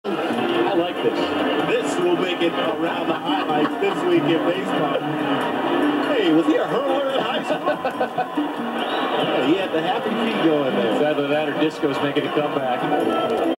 Dave Winfield Throws Bat Towards Pitcher Mike Henneman
Dave Winfield of the Minnesota Twins almost takes out relief pitcher Mike Henneman of the Detroit Tigers by throwing his bat right towards the pitcher during an at-bat! "Winnie" was famous for throwing his bat, and numerous occasions had fans at Yankee Stadium ducking for cover because of Winfield's bat always slipping out of his hands!